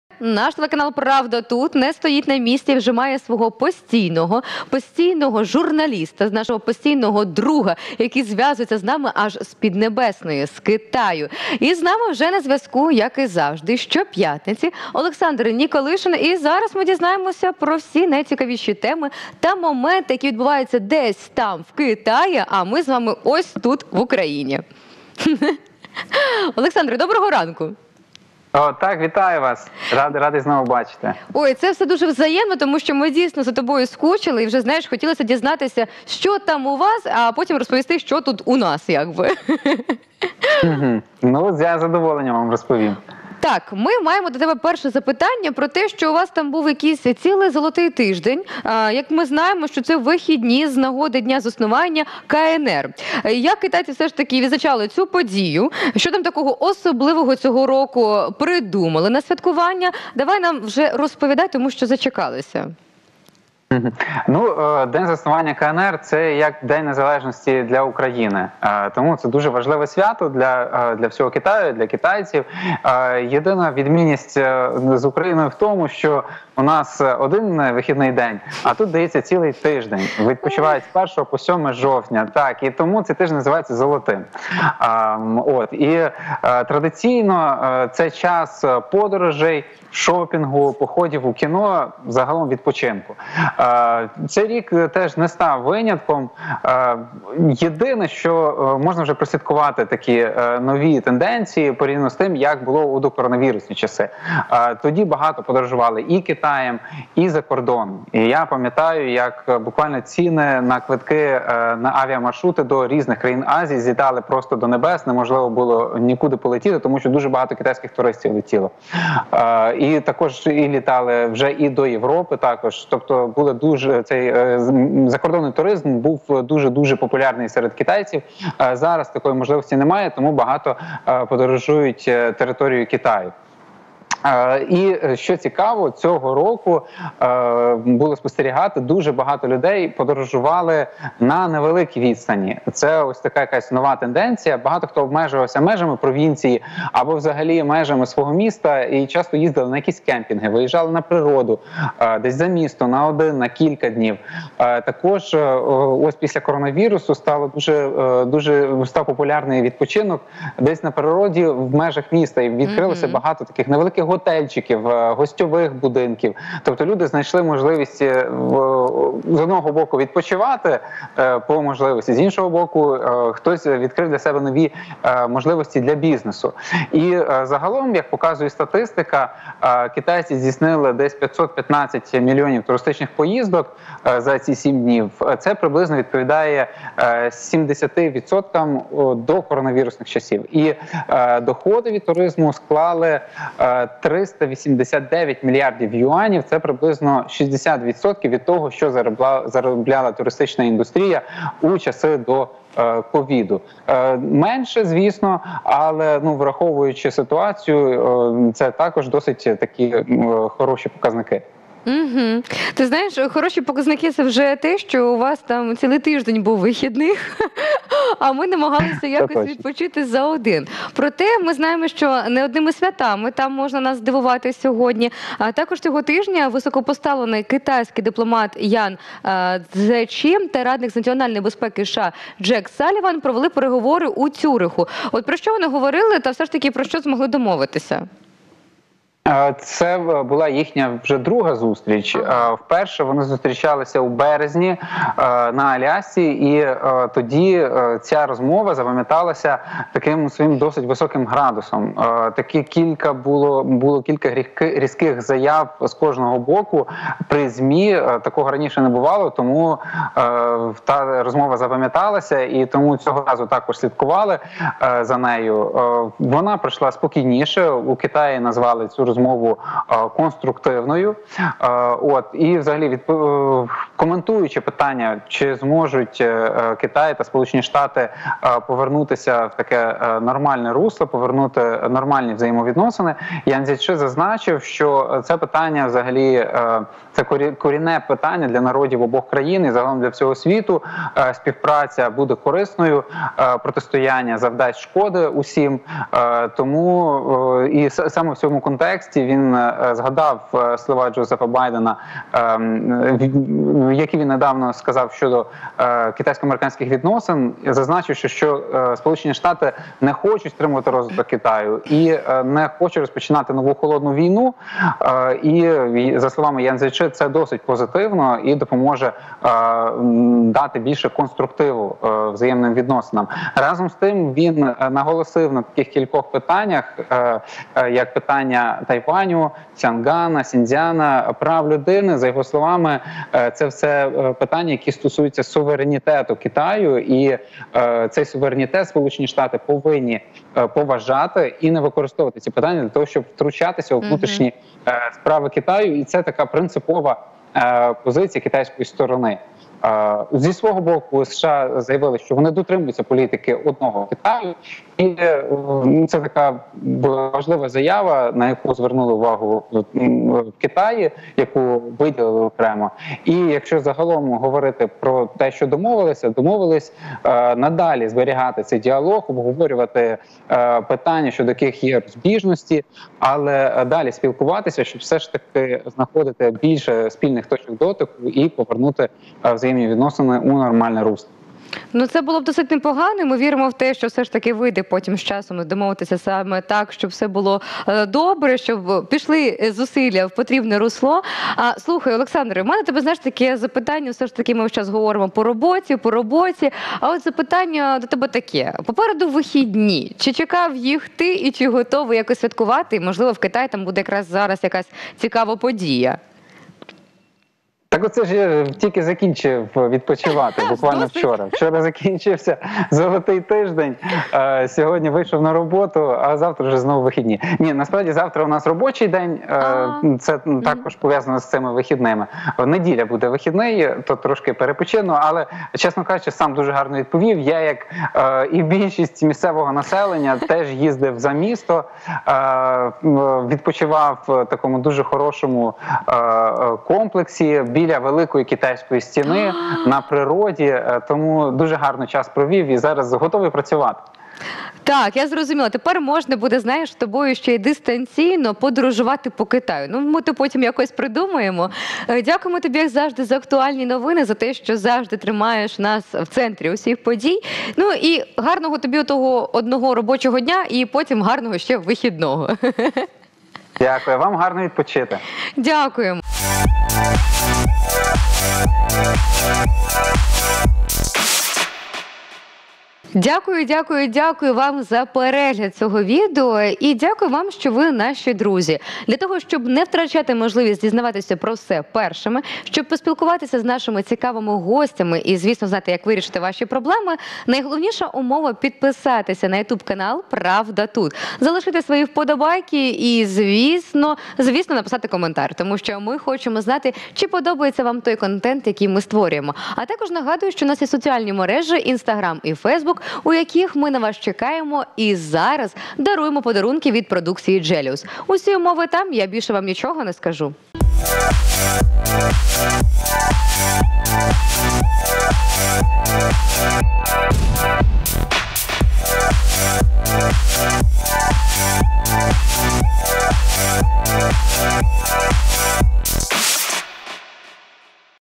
ПравдаТУТ LIVE: Олександр Николишин про актуальні новини КНР
На зв’язку з ранковою студією ПравдаТУТ LIVE співробітник китайської медіагрупи Олександр Николишин, який поділився найактуальнішими новинами останніх днів КНР. У Китаї завершився «золотий тиждень», а саме вихідні з нагоди Дня заснування КНР. Олександр розказав як китайці зазвичай проводять цей час і про особливі святкування цього року. Також Олександр розповів про перемовини високопоставленого китайського дипломата Ян Цзєчи та радника з національної безпеки США Джек Салліван у Цюріху і про що домовилися посадовці.